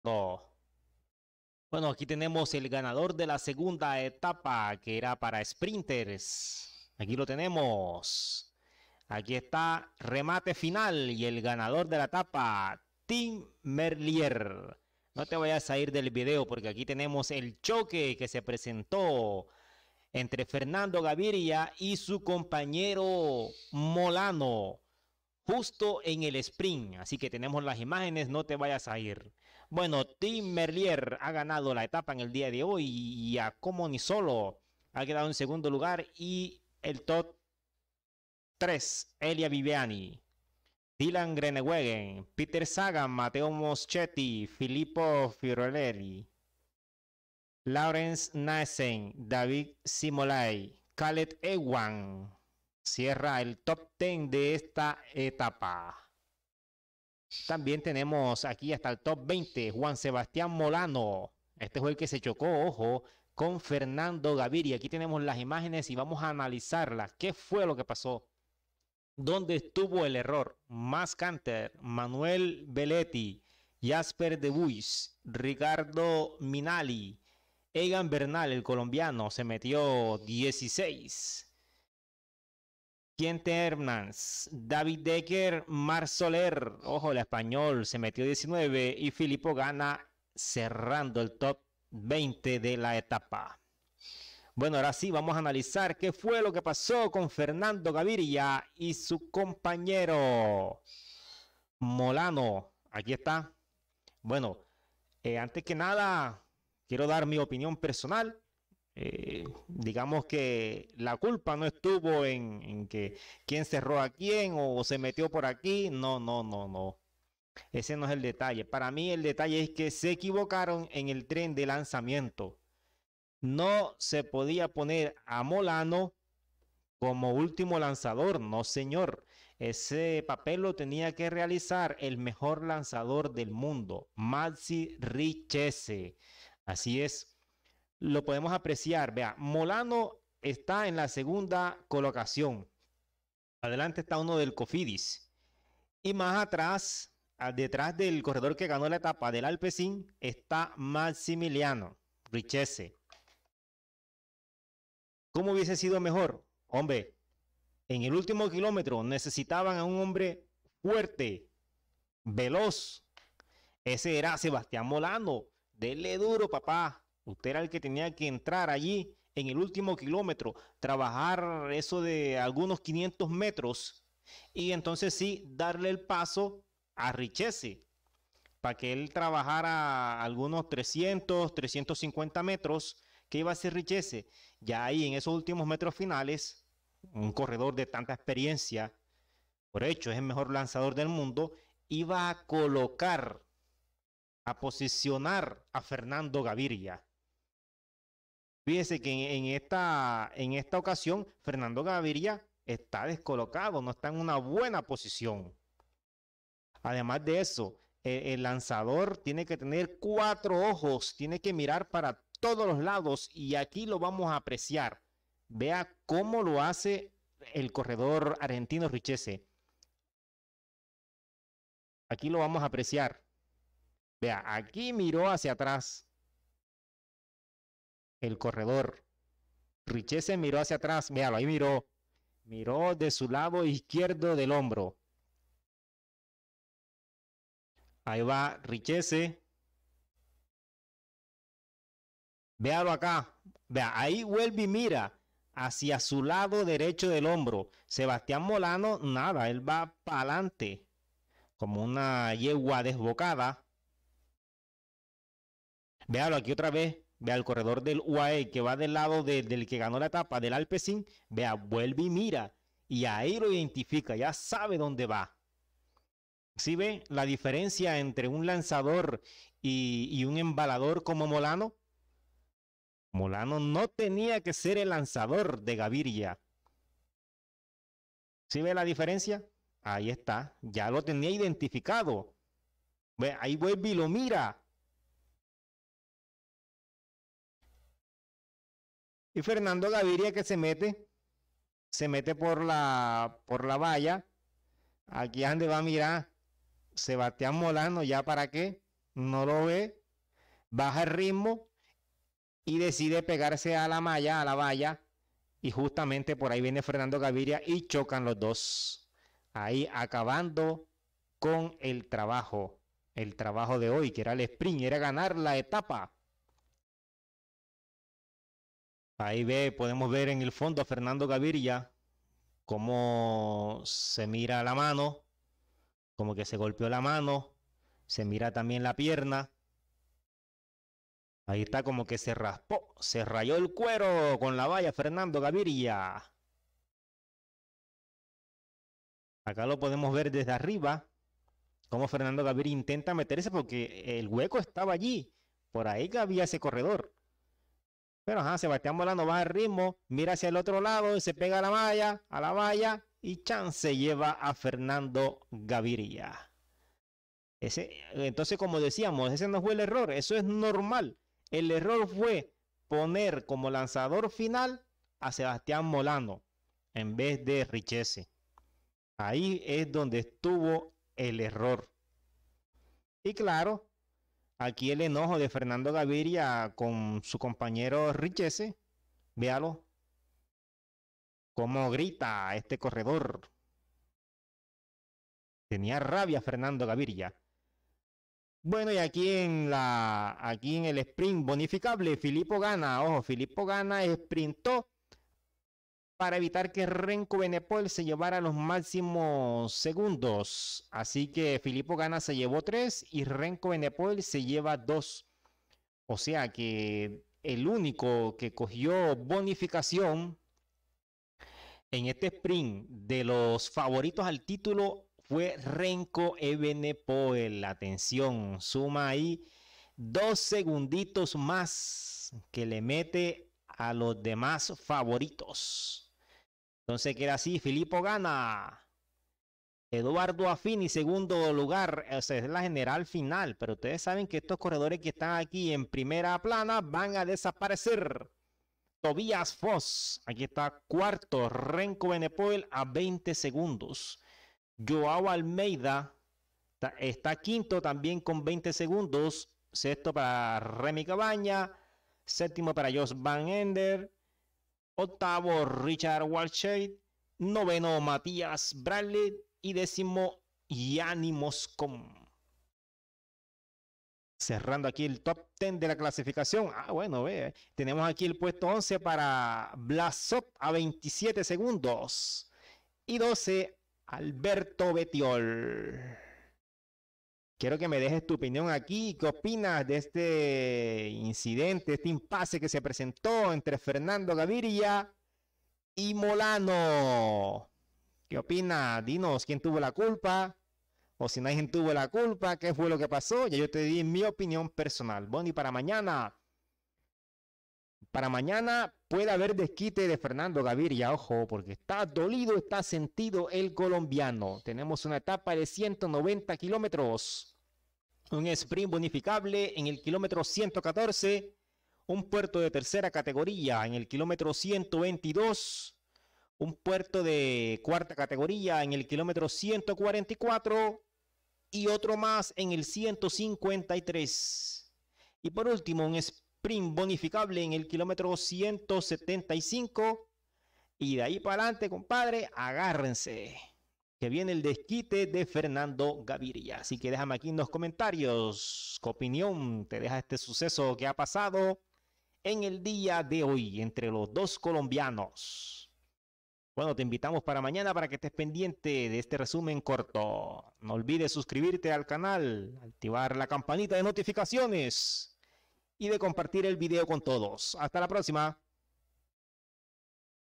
FERNANDO GAVIRIA Y MOLANO CHOCAN EN EL SPRINT FINAL. QUE PASO?
MIRA AQUI EL CHOQUE DE LOS COLOMBIANOS FERNANDO GAVIRIA Y MOLANO EN EL SPRINT FINAL. QUE PASO?